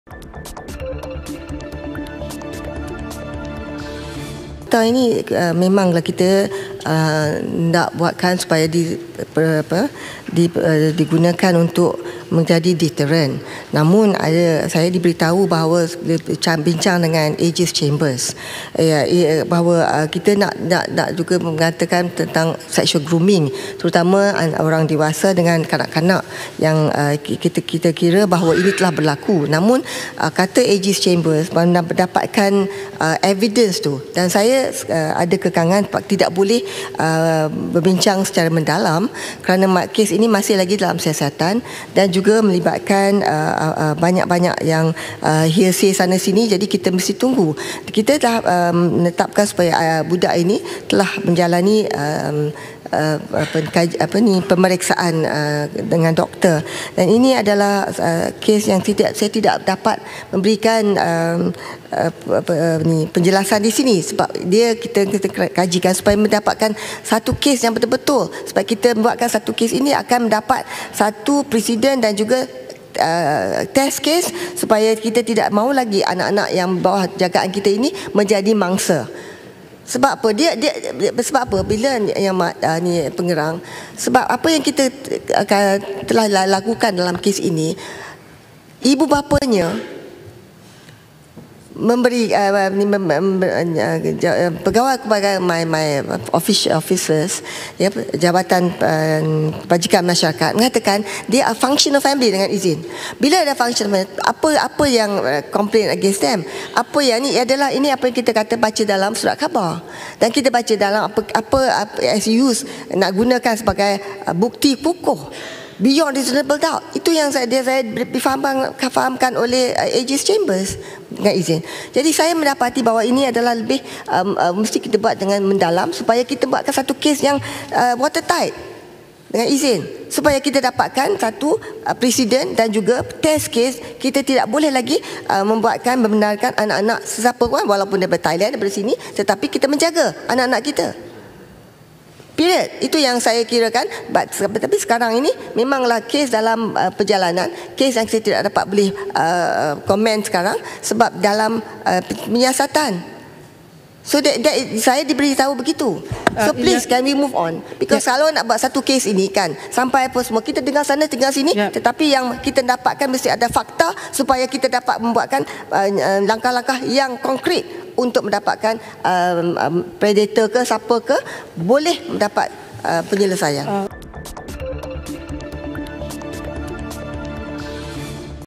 Child marriage: We need water tight case, says Wan Azizah
A thorough investigation is being carried out into the marriage of a 41-year-old Kelantan man to an 11-year-old girl to use the outcome as a test case, the Dewan Rakyat was told today.

Women, Family and Community Development Minister Dr Wan Azizah Wan Ismail said the investigation would look into all the angles.

Wan Azizah, who is also the Deputy Prime Minister, said the Attorney-General’s Chambers was heading the probe.

“We want to probe from all angles and have a water tight case,” she said when winding up the debate on her ministry.